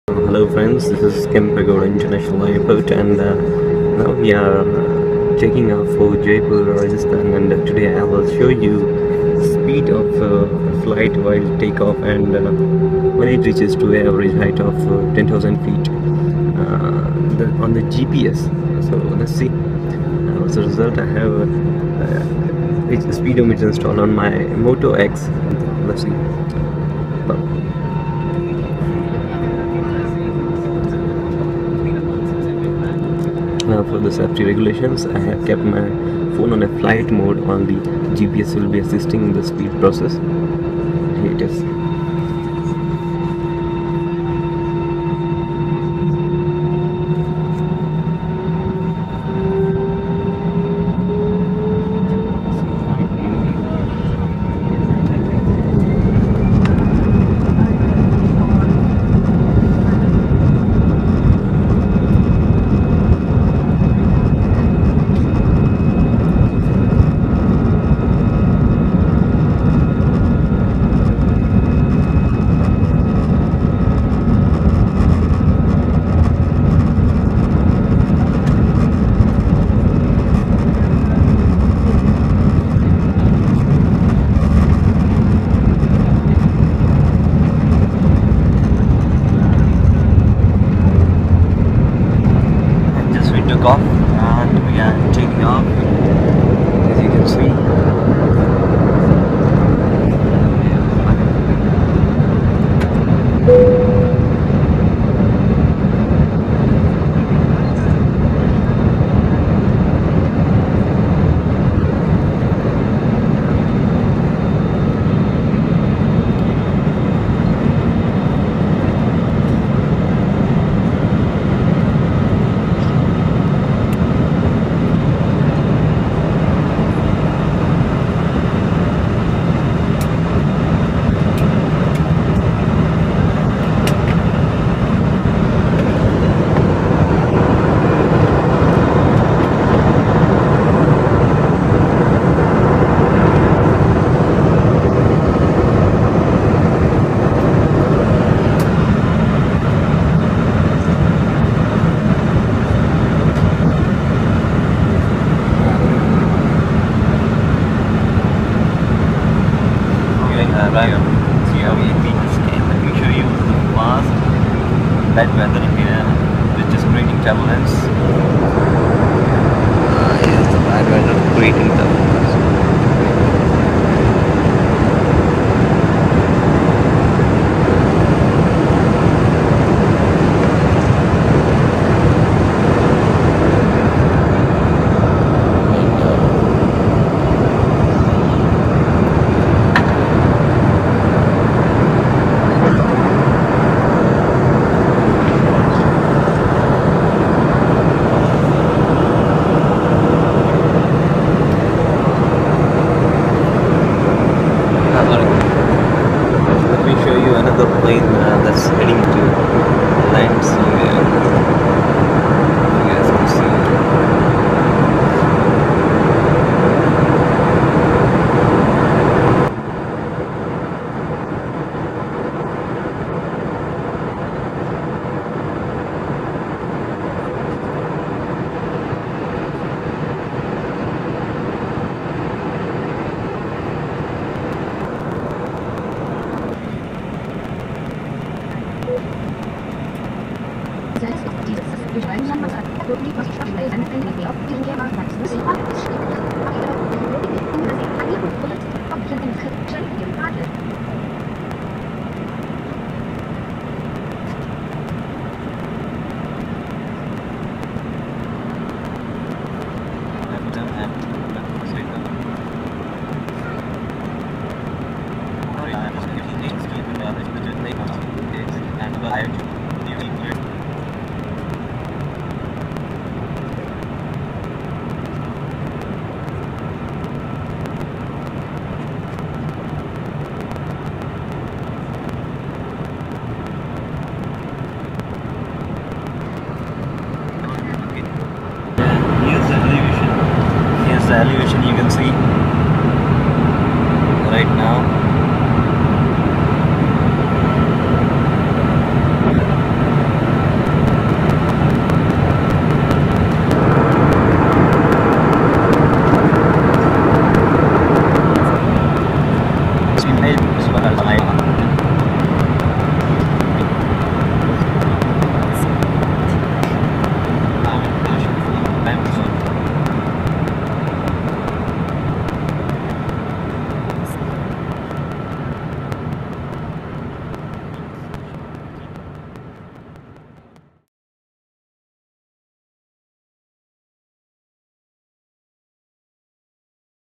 Speed and Elevation of Airbus Aircraft using Speedometer on Smartphone
Check out Takeoff like never before. AirAsia India Flight Takes off from Bangalore and we watch the speed and elevation via Smartphone. Meanwhile, we also have some amazing views and spottings of other Aircraft landing on same Airport as we climb.